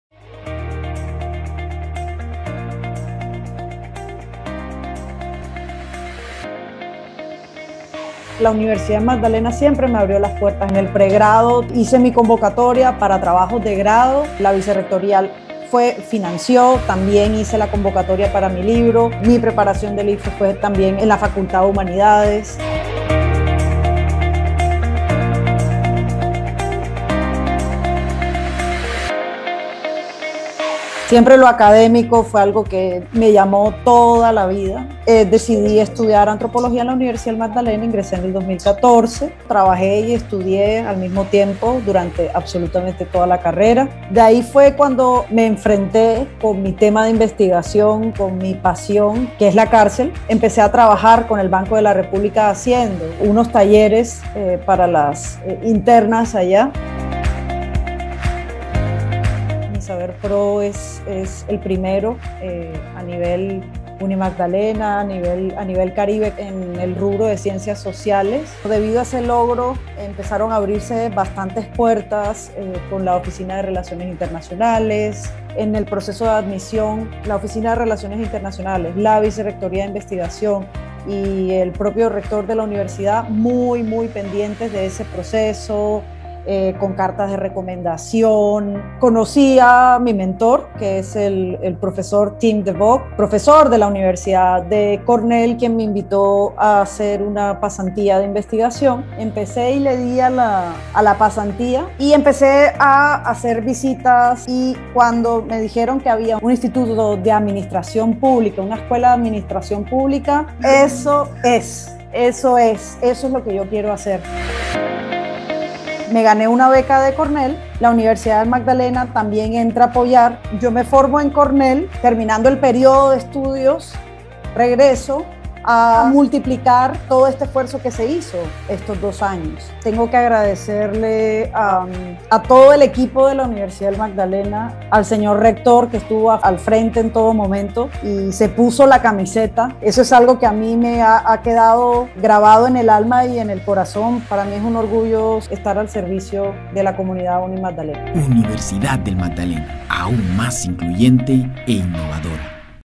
BA011_GRADUADA DE UNIMAGDALENA OBTIENE TÍTULO COMO MÁSTER EN CORNELL UNIVERSITY DE NUEVA YORK
La antropóloga Andrea Llinás Vahos es la primera profesional de la Universidad del Magdalena becada y titulada por la Universidad Cornell de Estados Unidos. El paso de Magdalena a Nueva York para realizar sus pasantías y conseguir el Máster en Administración Pública, fue respaldado por esta Alma Mater a través de una alianza que promueve la movilidad internacional de la comunidad académica. Su vida es un ejemplo de disciplina y grandes logros universitarios. ¡Conoce su historia!